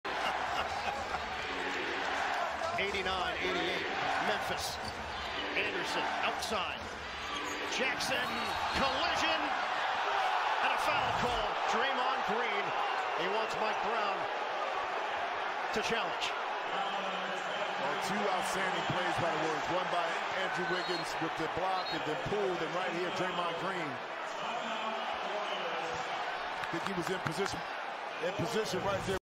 Draymond Green called for the blocking foul on a driving JJJ which would be his 5th;
Golden State Warriors vs Memphis Grizzlies